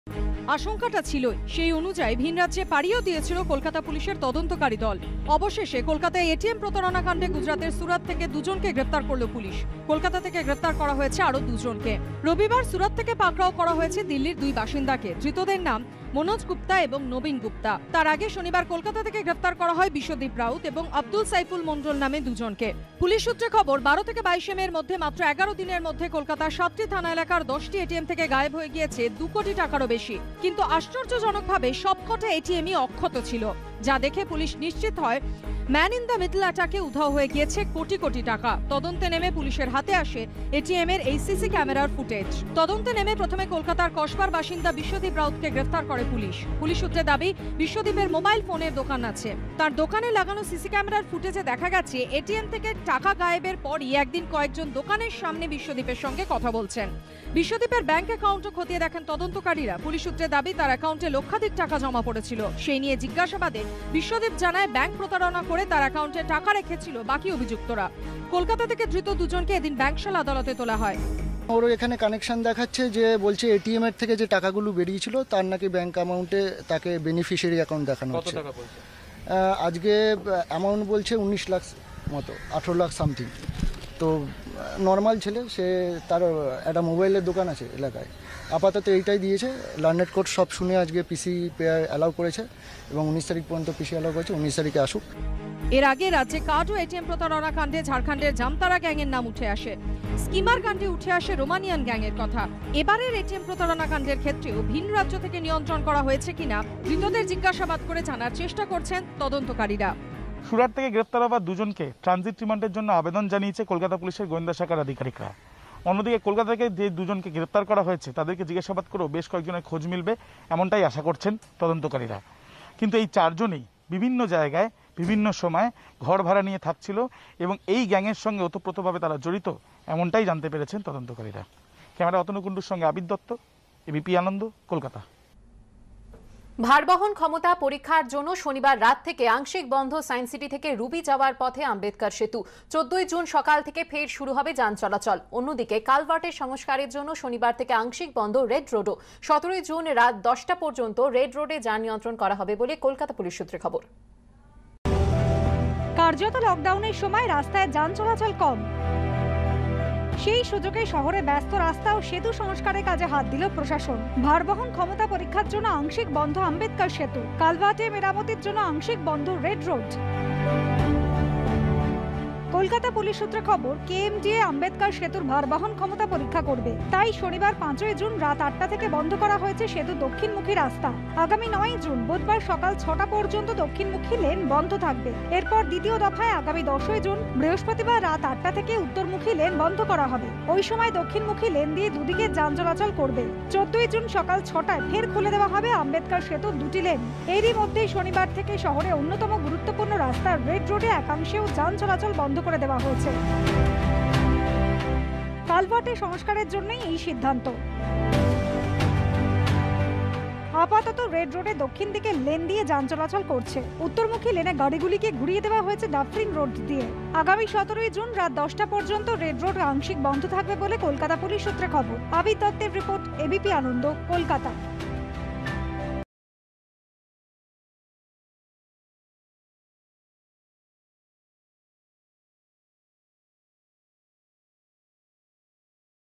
ATM Theft Arrest: পর পর এটিএম লুঠ, সুরাত থেকে দু'জনকে গ্রেফতার করল কলকাতা পুলিশ
কলকাতায় (Kolkata) একাধিক এটিএমে চুরির (ATM theft) ঘটনায় ভিনরাজ্যের যোগ থাকার আশঙ্কা করেছিল কলকাতা পুলিশ। সেই অনুযায়ী বেশ কয়েকটি রাজ্যে যায় কলকাতা পুলিশের বিশেষ দল। অবশেষে রবিবার গুজরাটের সুরাত থেকে এই চুরির ঘটনায় গ্রেফতার করা হল ২ জনকে। ধৃতরা দিল্লির বাসিন্দা। শনিবার কলকাতা থেকে গ্রেফতার করা হয়েছে আরও ২ জনকে। এটিএম-গুলির সিসিটিভি ফুটেজের সূত্র ধরেই ওই ৪ জনকে গ্রেফতার করা হয়েছে।